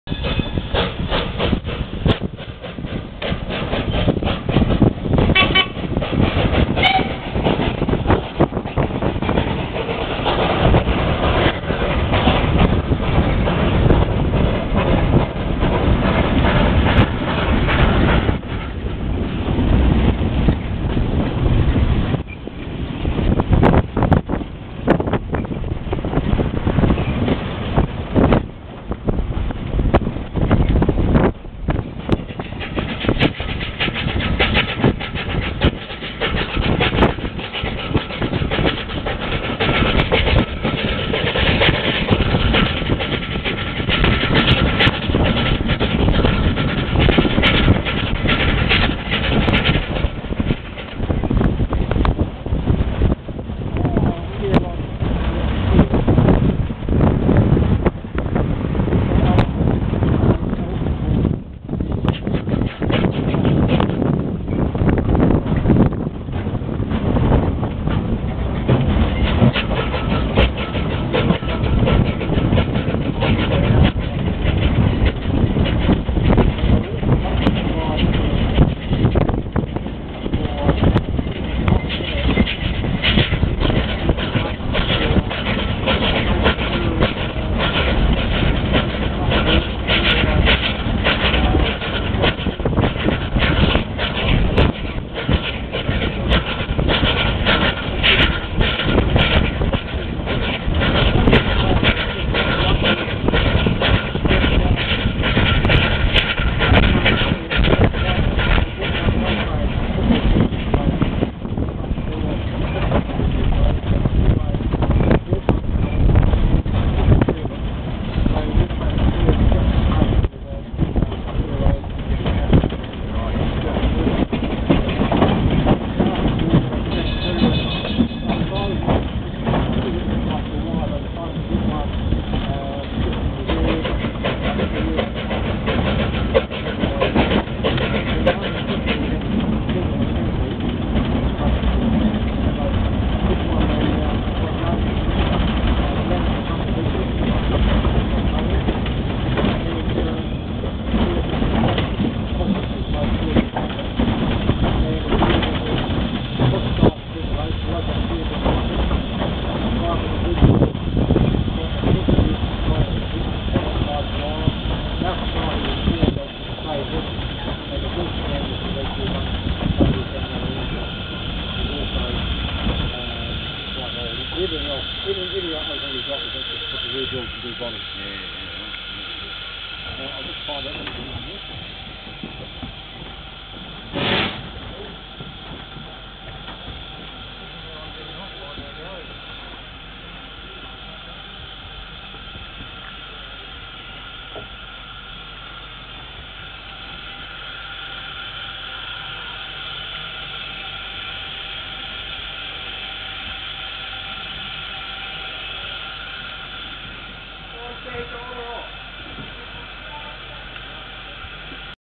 Riding a Steam tram around Ferrymead historical park. Thursday 24 November 2011
Travelling around the Ferrymead historical park on the top deck of a Steam tram. We pass a preserved trolley bus waiting for us to pass at an internal level crossing. Thursday 24 November 2011